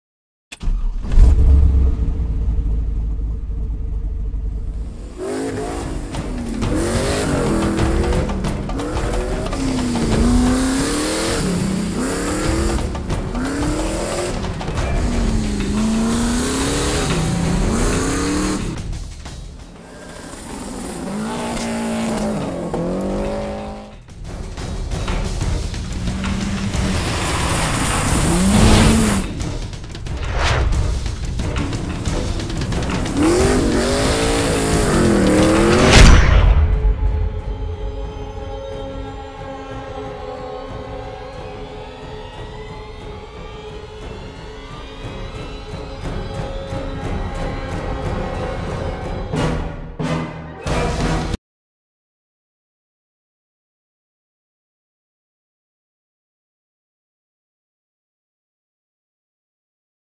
Heyne DRIVE Plugin Cinema 4D / Road Rage /Test Drive No.01 / CGI 3D Animated Short
Idea and Realization by: Opa Grampz / OldFartStories
Created with: MotionBuilder/ Cinema 4d / After Effects / Premiere Pro / Audition